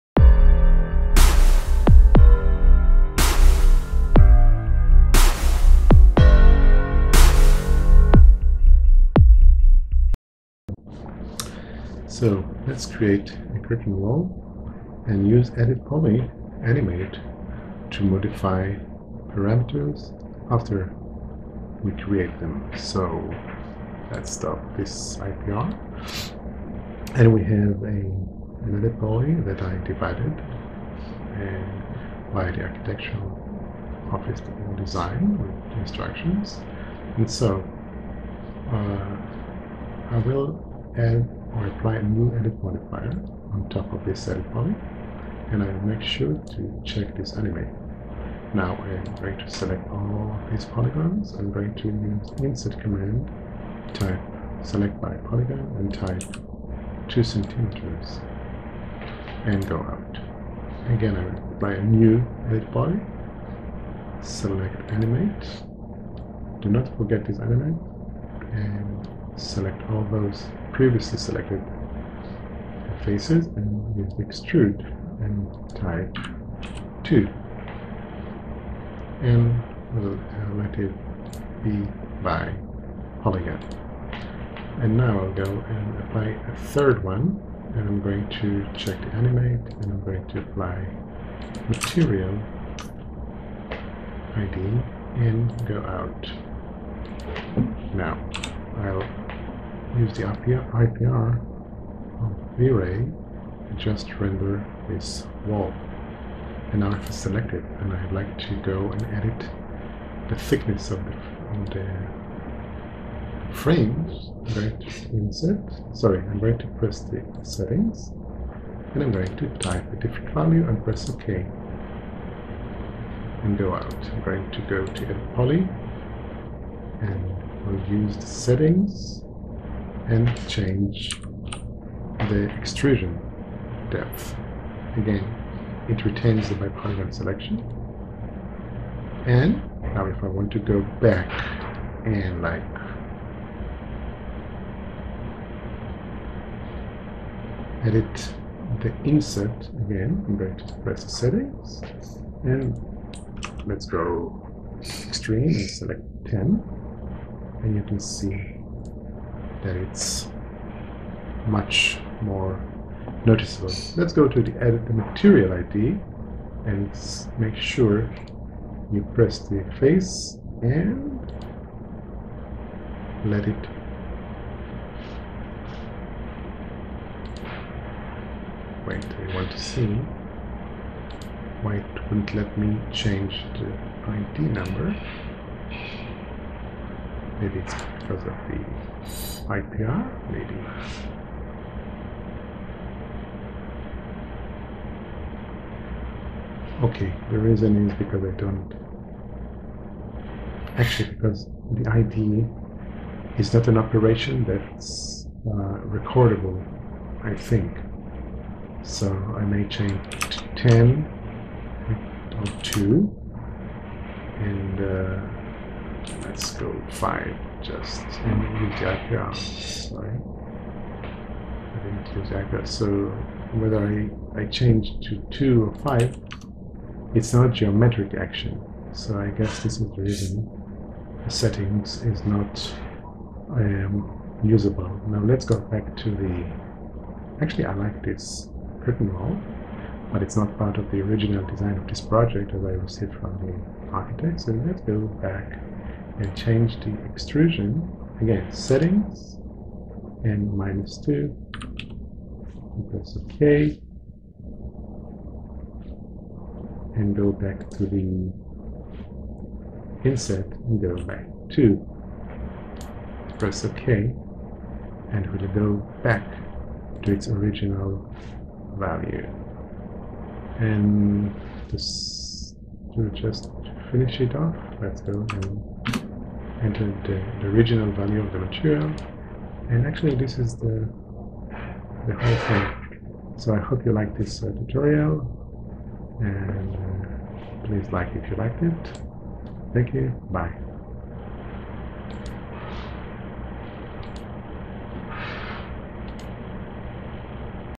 3ds max: Editpoly animate: changing parameters after the fact
A short tutorial about using edit poly modifier to allow for changes to be done after the fact.